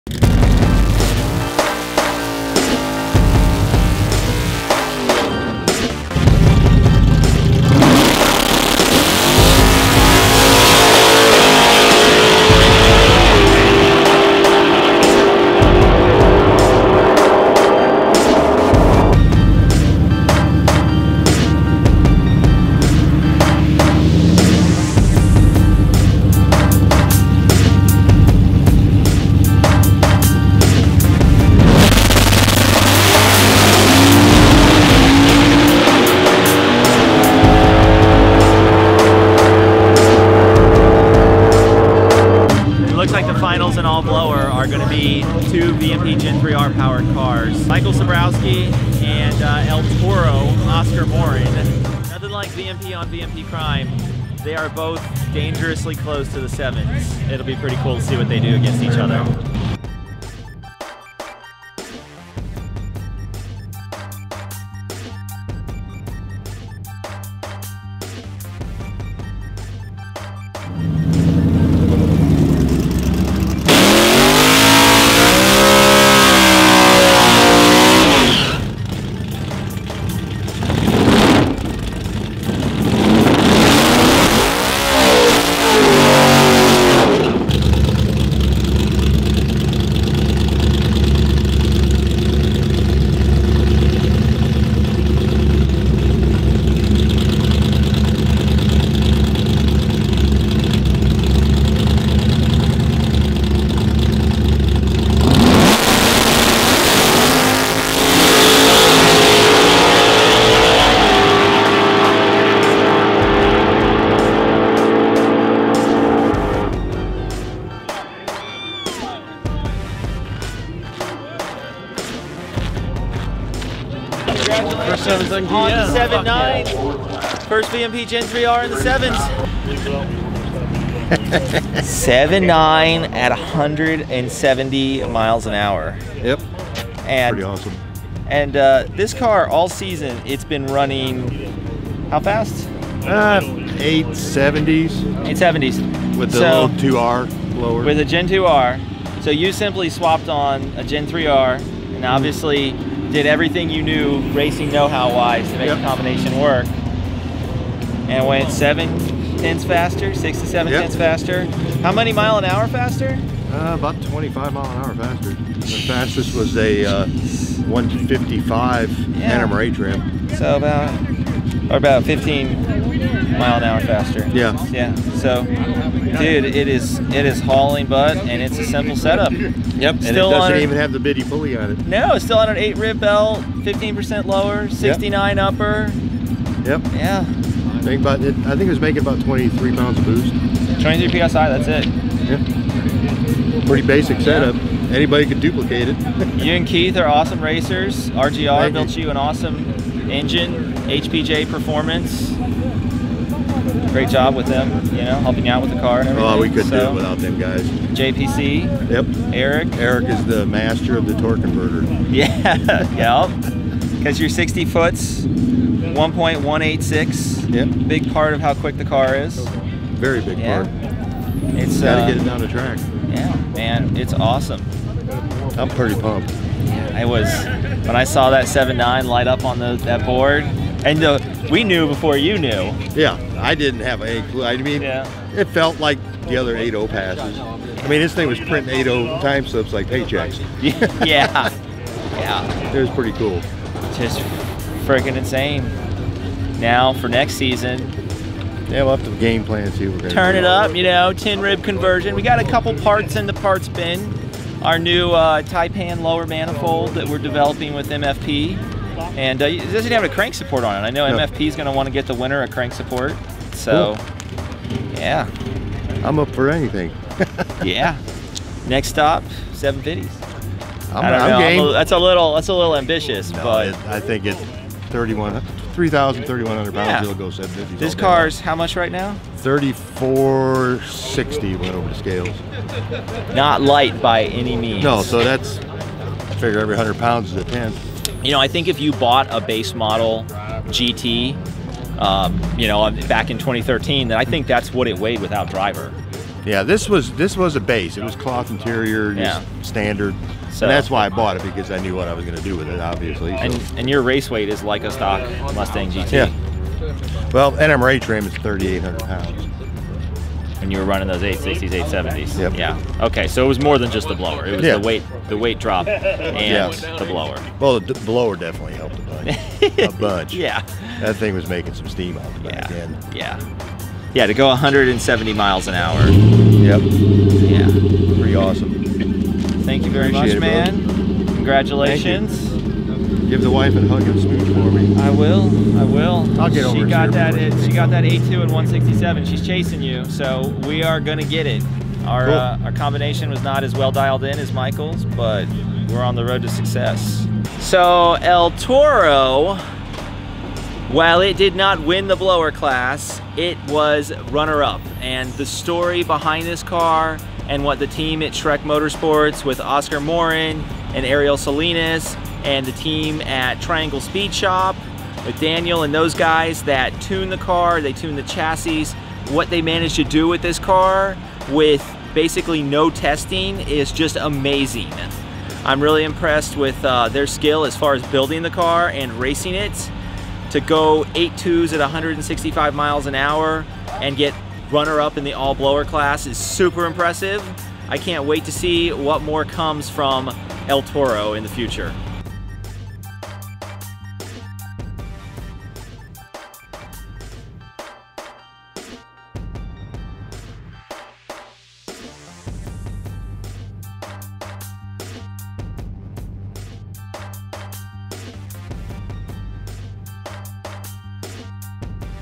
First VMP Gen3R in the 7's!
Mod Motor Nationals 2019 - VMP/ Magnuson Superchargers All Blower Finals

There was a ton of good racing at  Mod Motor Nationals this year. Held at South Georgia Motorsports Park, many customers with amazing builds showed up to represent VMP. The VMP/ Magnuson Superchargers All Blower class was full of all types of superchargers from VMP and our competitors but, in the end, it was VMP vs VMP in the finals for an exciting race!

See who came out on top and what the winning combination was!